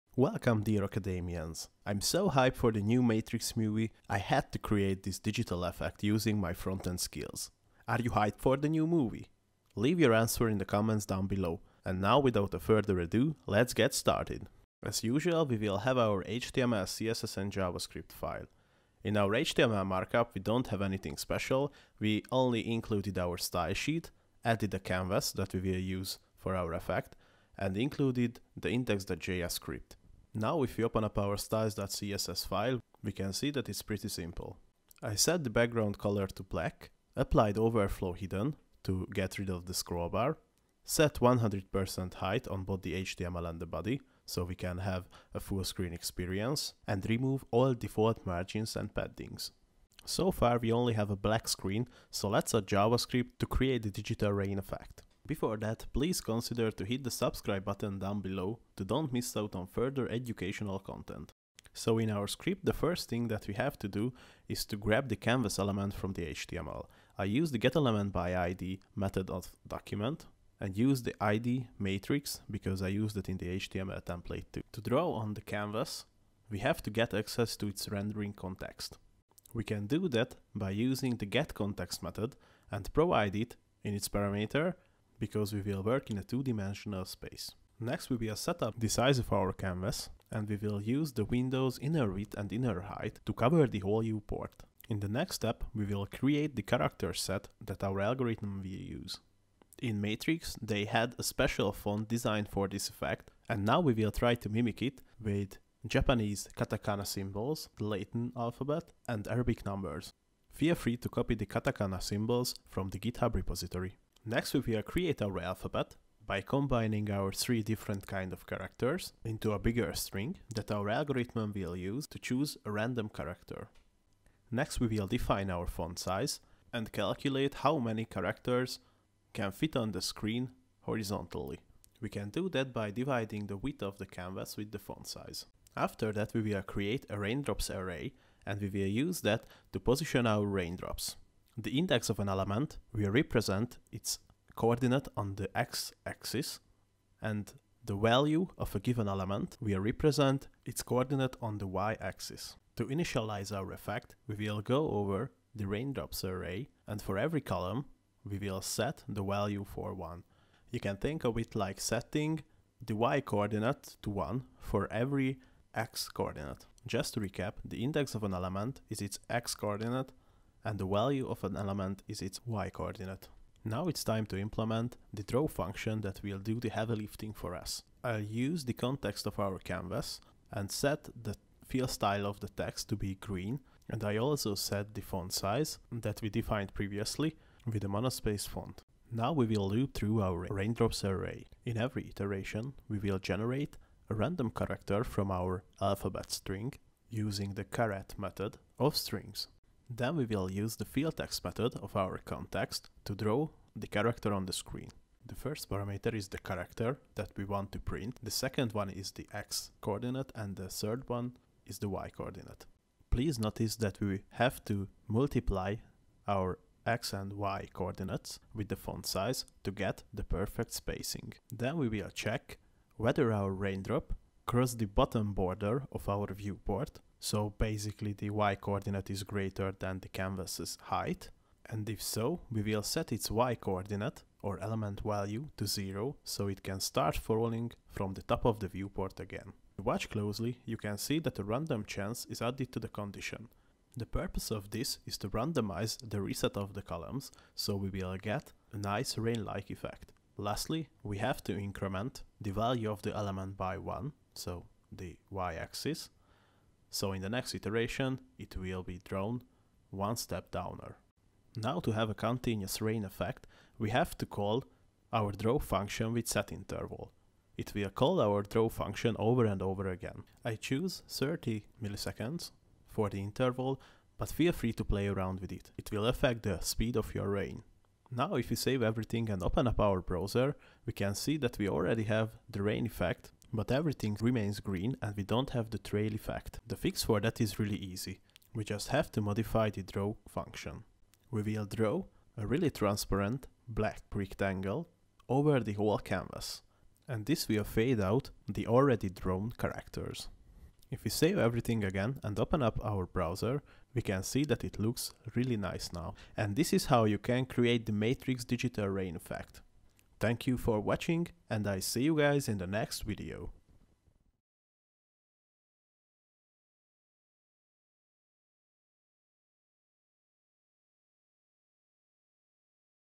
Matrix raining code effect using HTML, CSS and Javascript
I love Matrix, so I decided to recreate the iconic raining code effect using my frontend skills. I picked the easiest implementation so I think it is really well suited for beginners. I hope you will enjoy this project as much as I did.
Have fun!